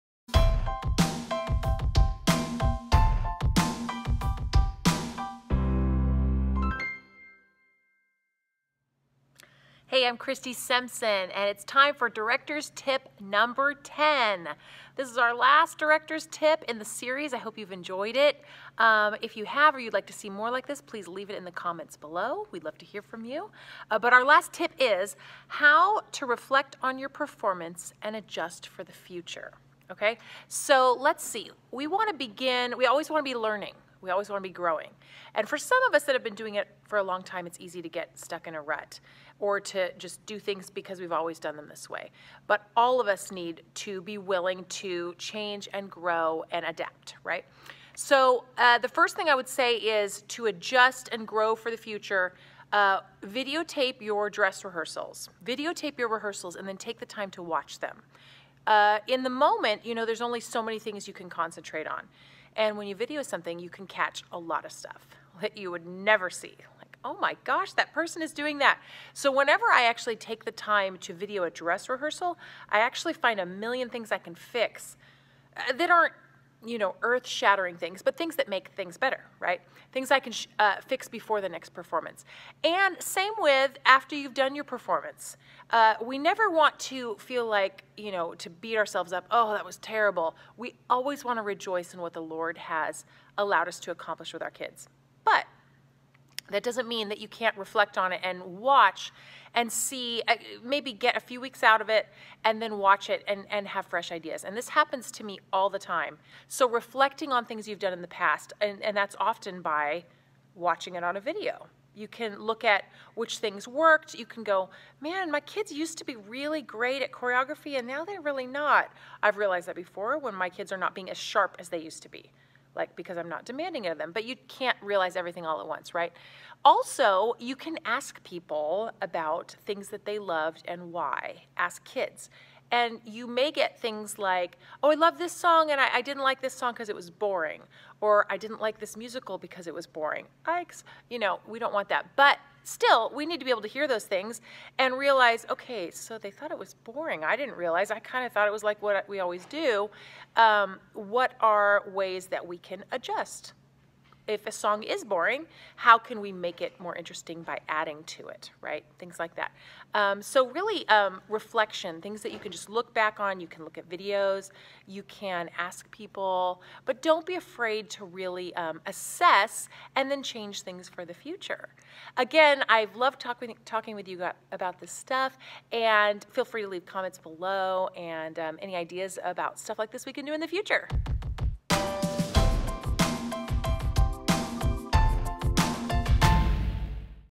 Director's Tip #10: How to reflect on your performance and adjust for the future
This week on Top Ten Director's Tips, Christy gives her FINAL tip...How to reflect on your performance and adjust for the future! Plan and simple, no performance is perfect. There is always room for us to learn and grow! What's the best way to do that? Watch now!